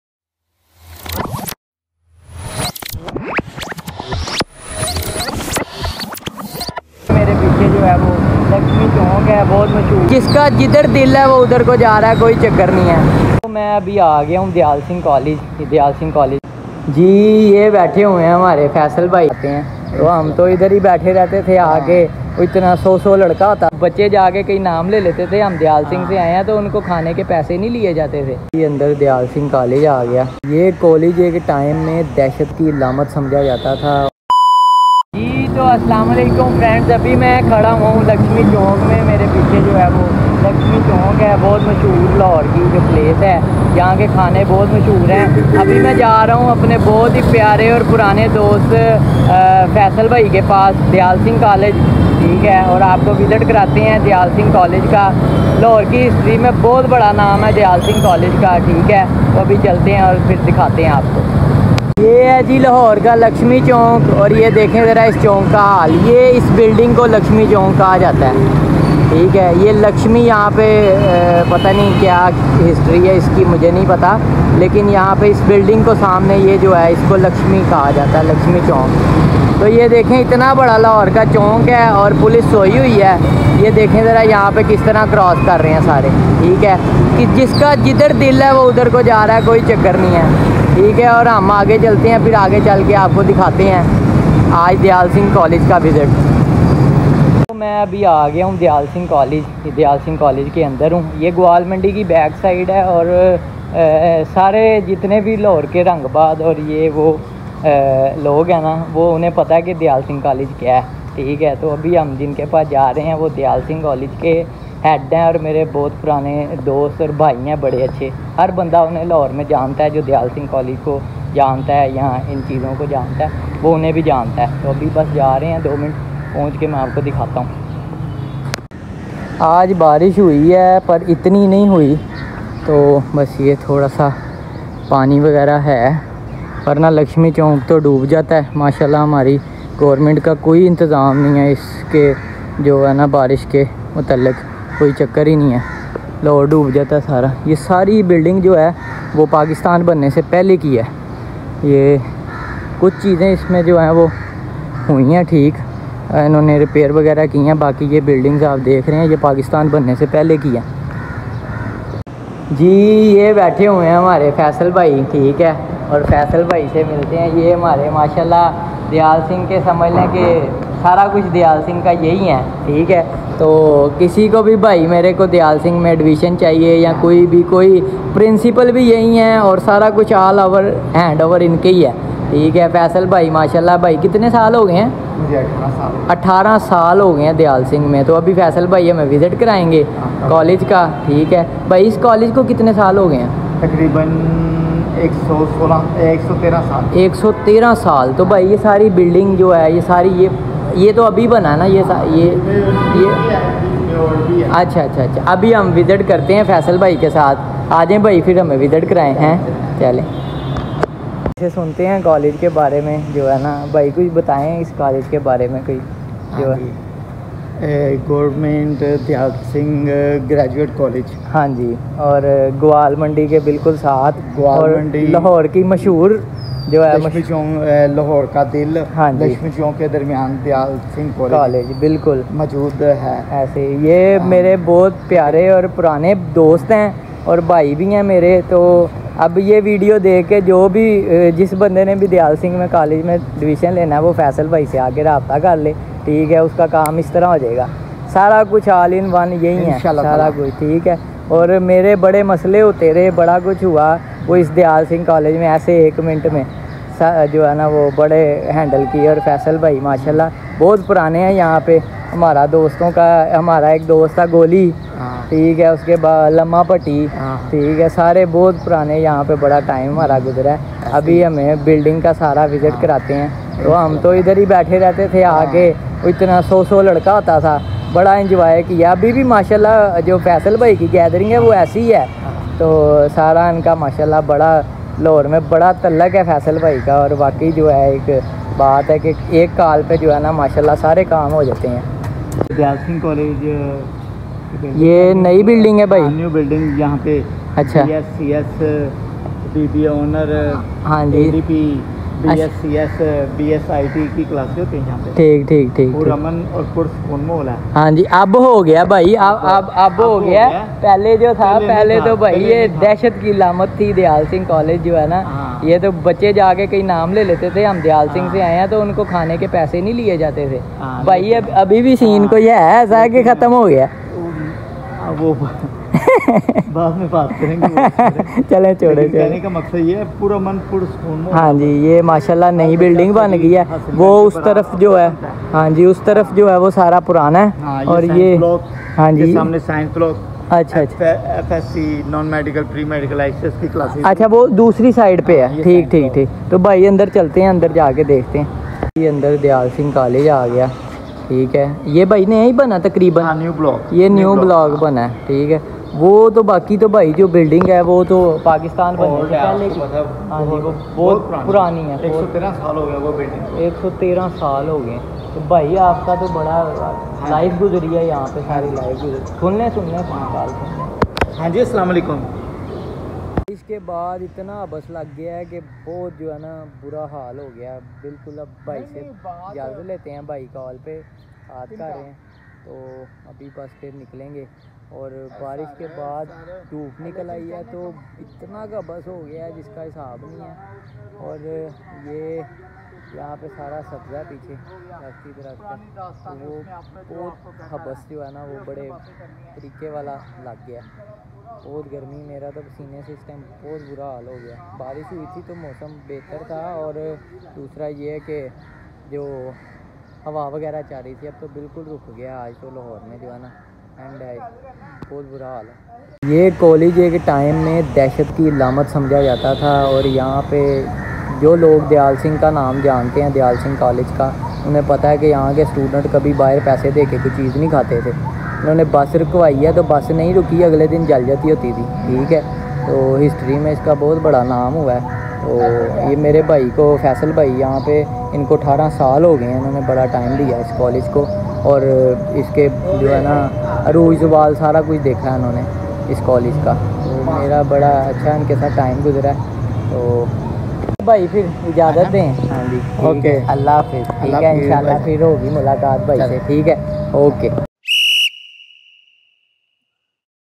Diyal Singh College | Gawalmandi Lahore
Mray purnay dost faisal bhai sy mulaqat ki jo k Diyal Singh College my 18 Sal sy Union Chala ray ha mara both waqt guzra ha is college may